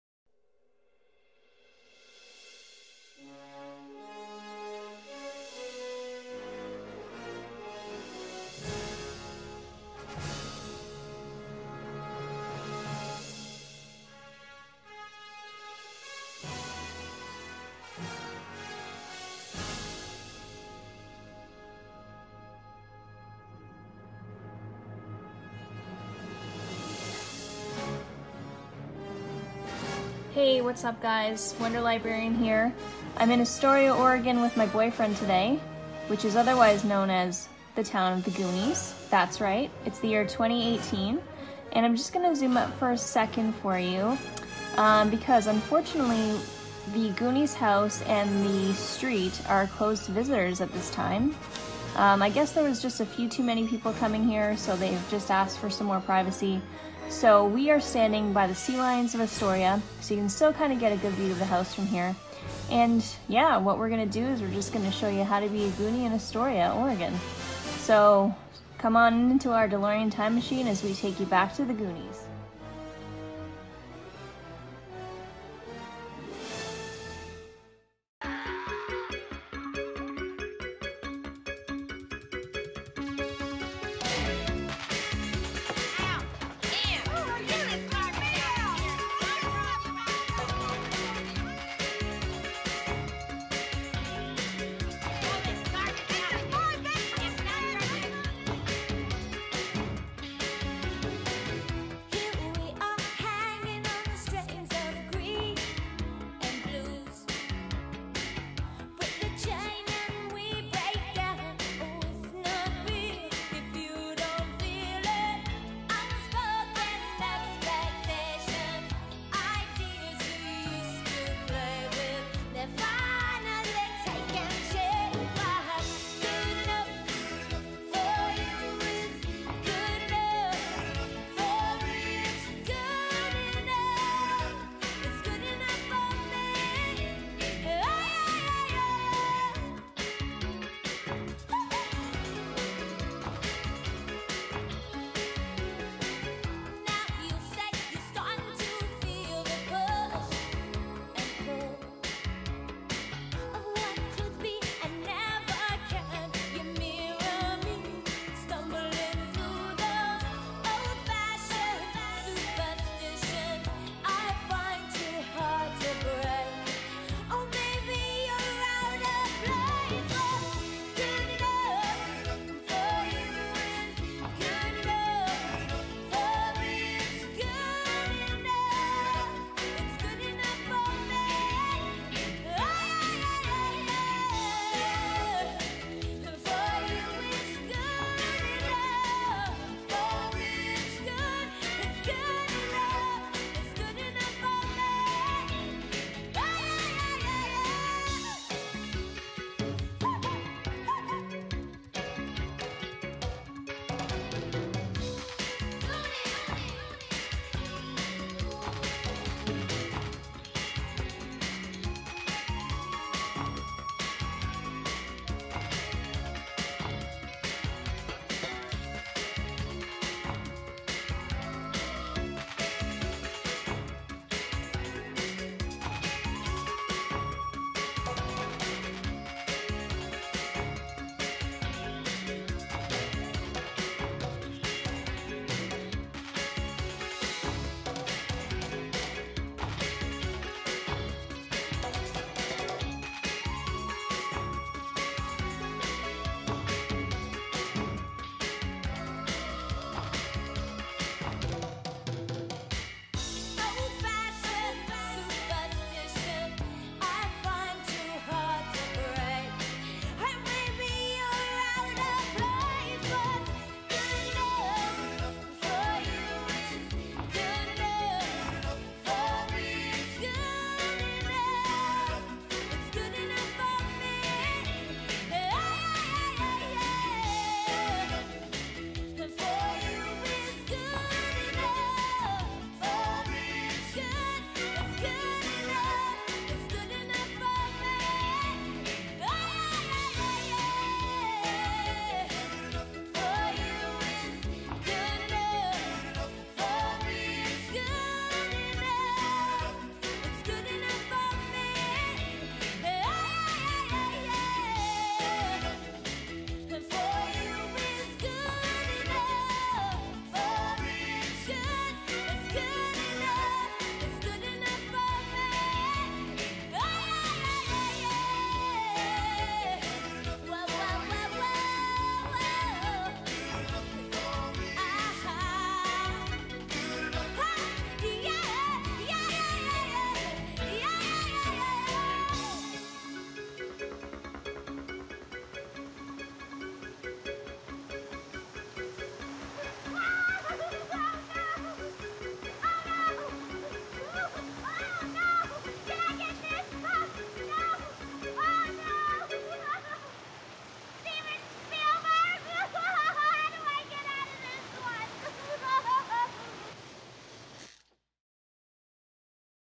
Back to the Goonies
My boyfriend Andrew and I went BACK to the Goonies in 2018! Most things haven't changed and it was AMAZING for us 80s kids to see!

Come along in our DeLorean Time Machine and learn how to be a Goonie in Astoria!